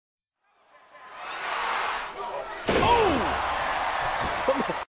BRW Stop Motion Animation #2
Finlay misses a turnbuckle clothesline on John Cena.